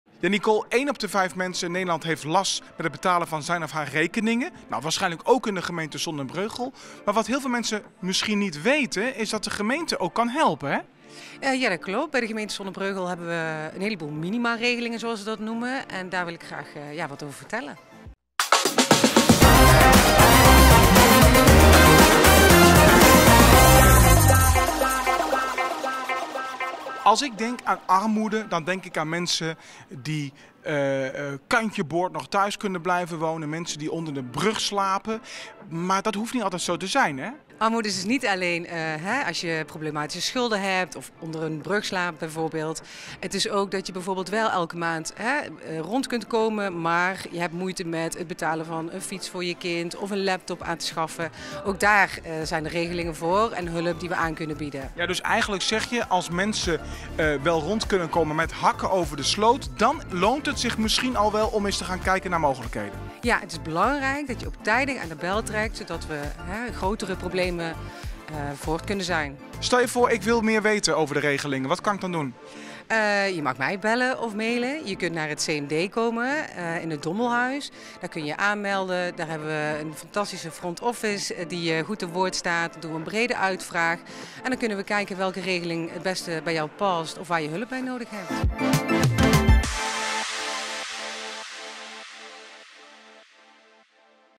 Regelingen voor hulp bij rondkomen
Voor inwoners die moeite hebben met het betalen van hun rekeningen hebben wij als gemeente diverse regelingen. Collega Nicole legt er meer over uit.